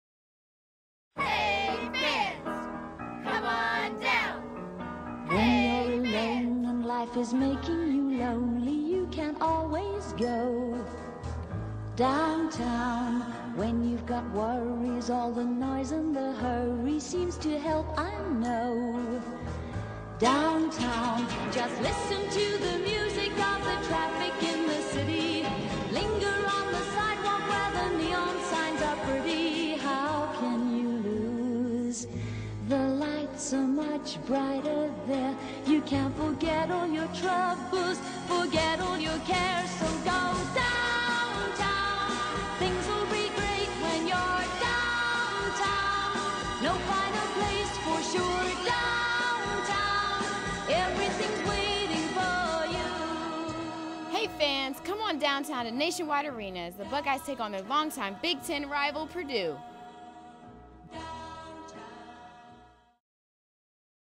Ohio State Women's Hoops at Nationwide Arena - Feb. 10, 2011
Head downtown for a showdown with Purdue! Buckeyes' archrival in women's hoops hits up Nationwide Arena for a late-season Bucks-Boilers Big Ten battle.